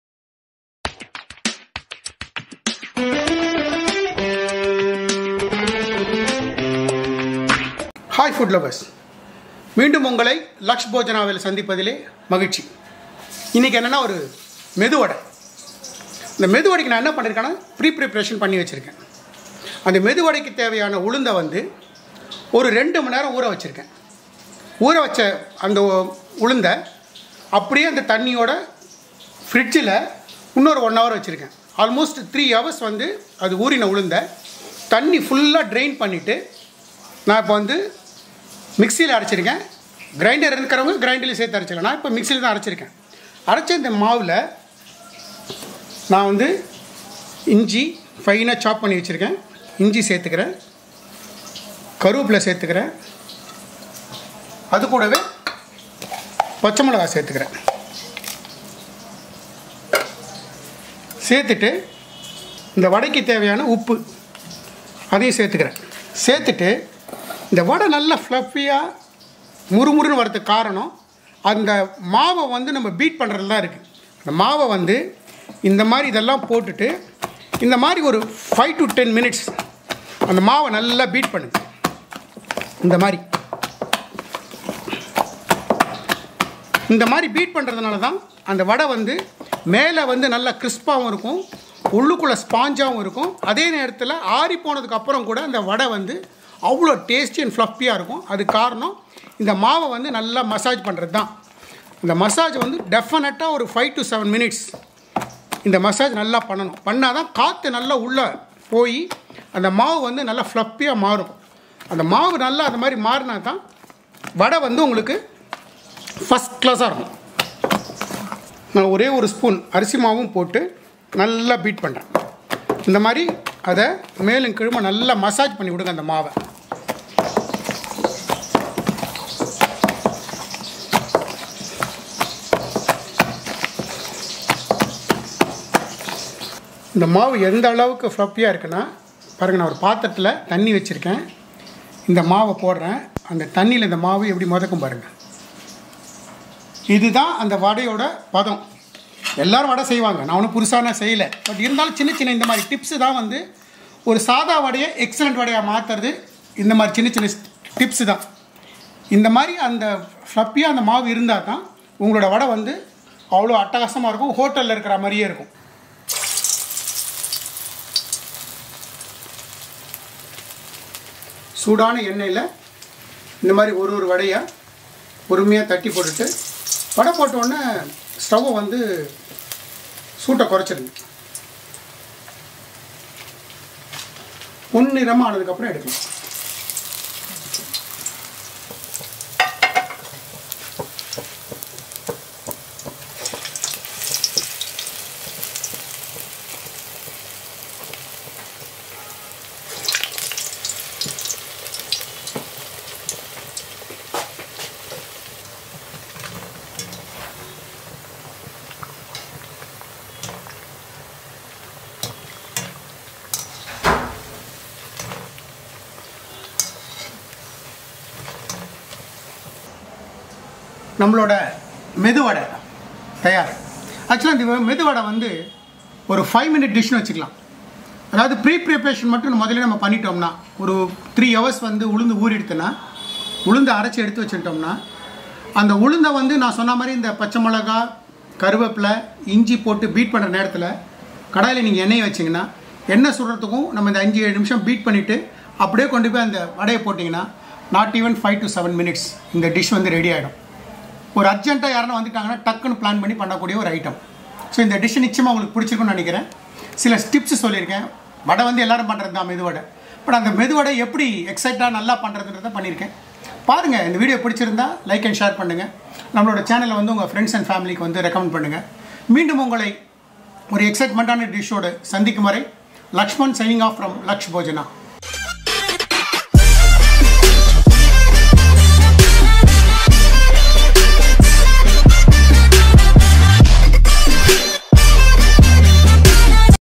எண்ணெய் குடிக்காத சுவையான மொறு மொறு மெது வடை இப்படி செய்யுங்க | Medhu Vada Recipe | Crispy Vadai
Hi 👋 This is Lakshman ❤️ from Laks Bhojana ☺️ Here is my Next video 😀

5 Minute Dish | Crispy Medu Vada Recipe at home | Ulundu Vadai | 5 Hours Preparation | மெது வடை - Episode 29